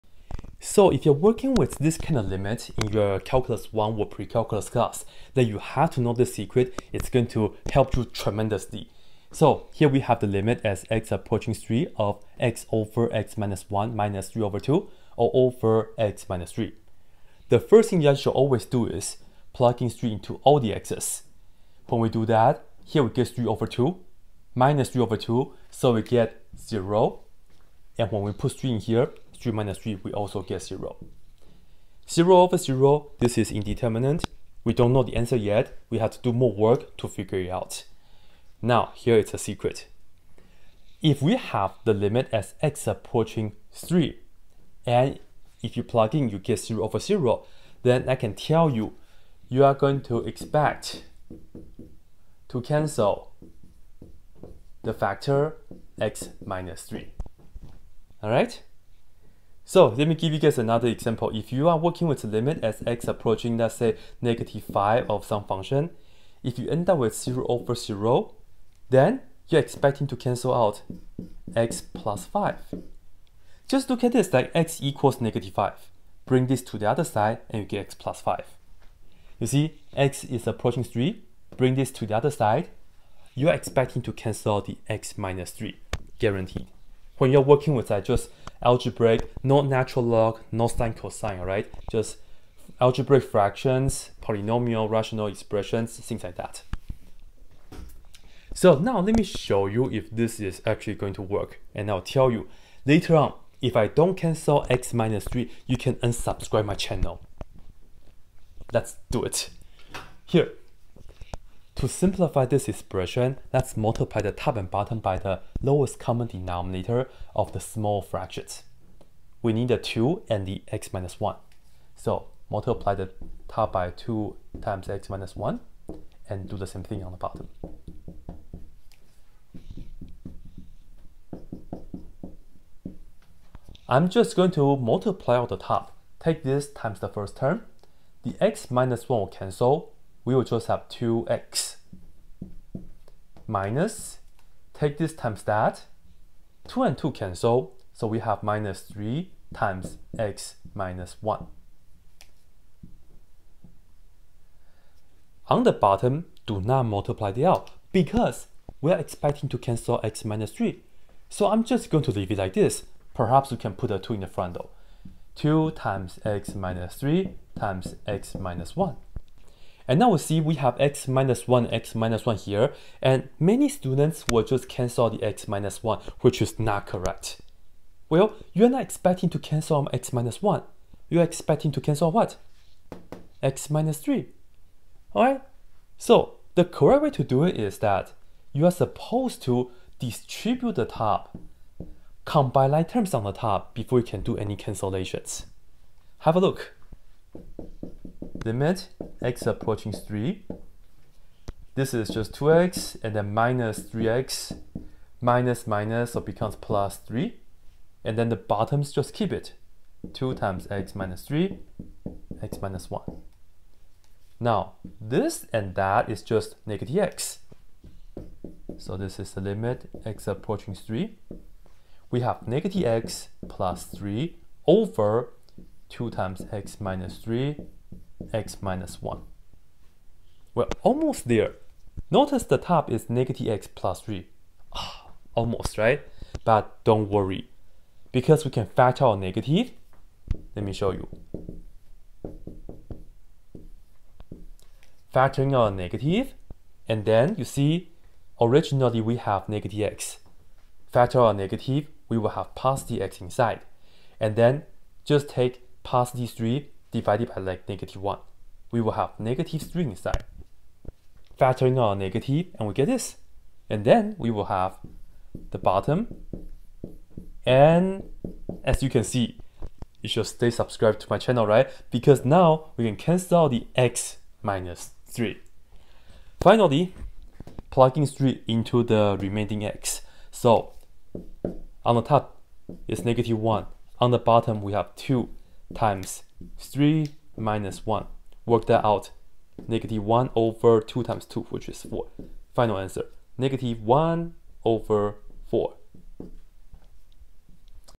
A *secret* for evaluating limits algebraically with 0/0 indeterminate form! Calculus 1 tip
Here's a tip when evaluating a limit with the indeterminate form of 0/0. This will help you with your Calculus 1 class when you are going over solving limits algebraically.